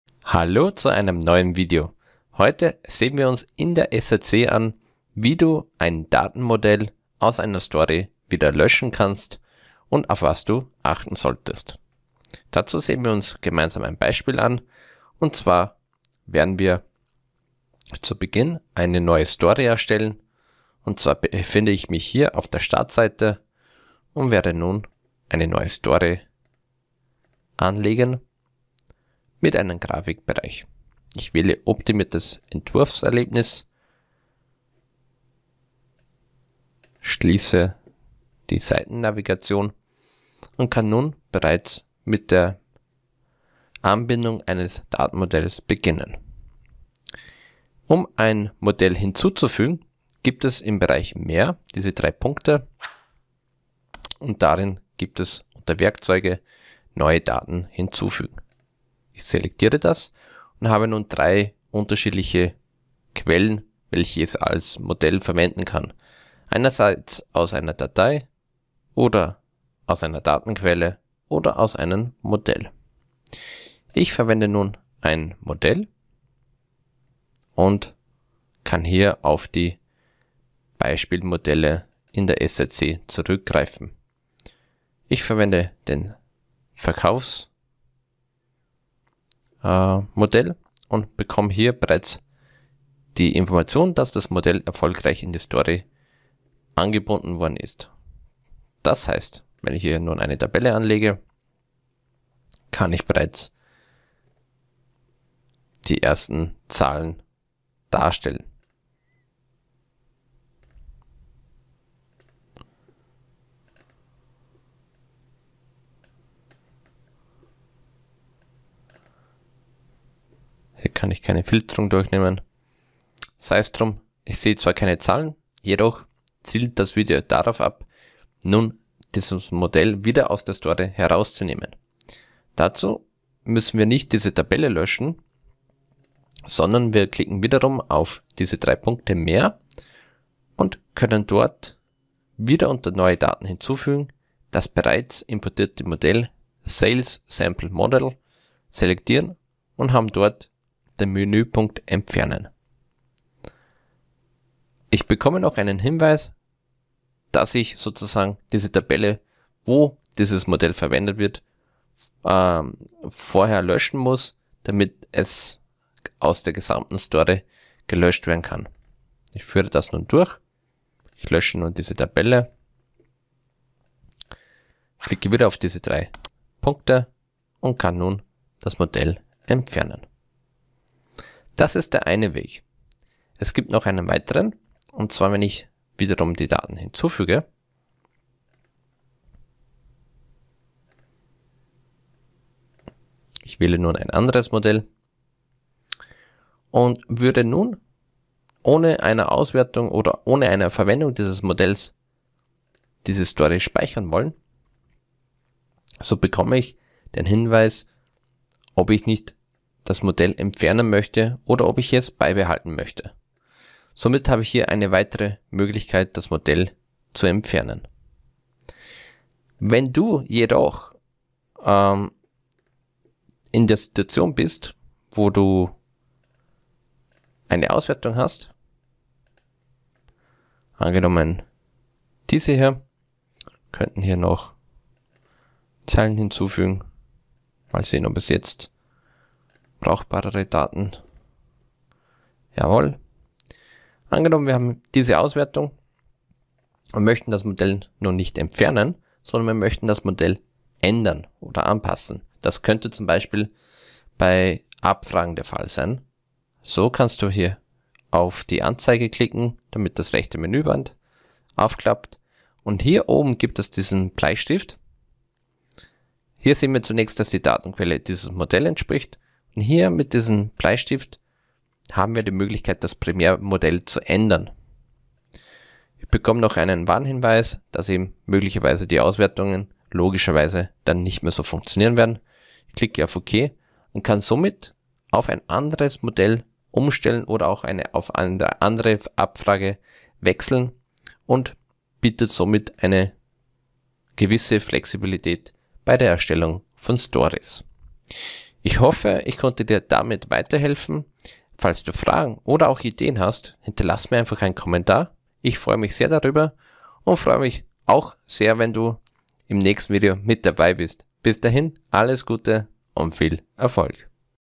Entfernen eines Modells aus einer SAP Analytics Cloud Story! 🚩
Das Löschen eines Modells in einer SAP Analytics Cloud Story ist ein wichtiger Schritt, um Berichte und Analysen zu aktualisieren und auf dem neuesten Stand zu halten. Durch das Entfernen eines nicht mehr benötigten Modells können Benutzer die Datenquellen verwalten, die Präsentation ihrer Geschäftsdaten verbessern und die Effizienz in der Datenanalyse steigern. Diese Anleitung bietet einen Überblick darüber, wie Benutzer Modelle in ihren SAP Analytics Cloud Stories löschen können, um ihre Arbeitsabläufe zu optimieren und den Fokus auf relevante Informationen zu lenken.